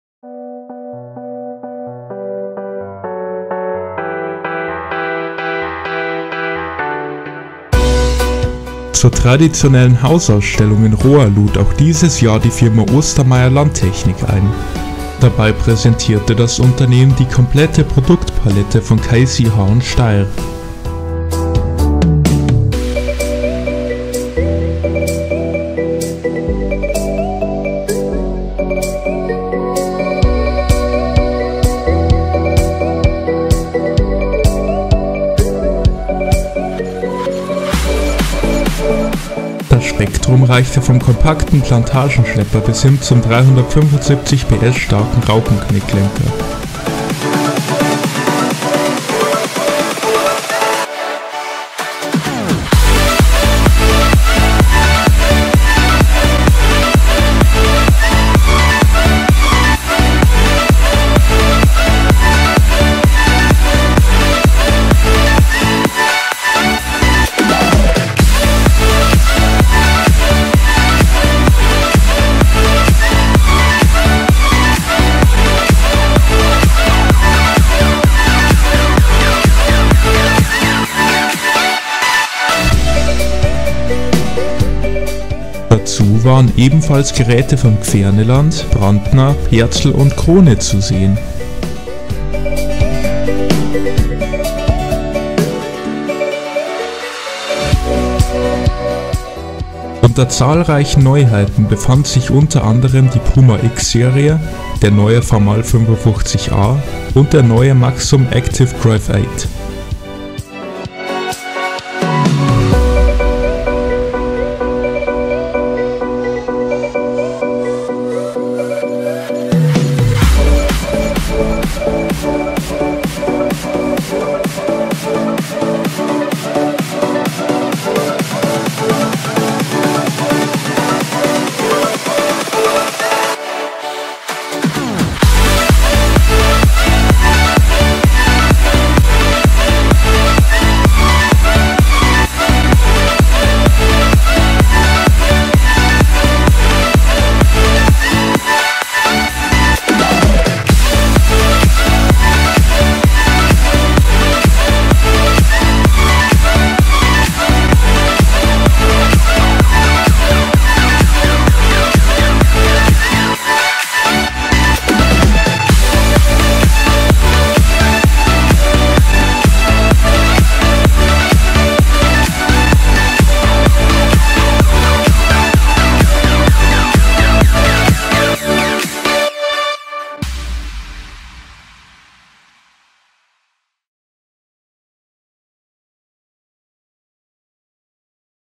Die Case IH Produkte auf einen Schlag - Ostermayr Hausausstellung 2018
Am 27.Mai lud die Firma Ostermayr Landtechnik zur traditionellen Hausausstellung in Rohr ein! Dabei waren neben zahlreichen Traktoren von Case IH und Steyr auch Produkte von BSA, Brantner, Perzl, Kverneland und Krone zu sehen.

Kamera: Canon IXUS 125 HS
Bearbeitung: Sony Vegas Movie Studio HD Platinum 10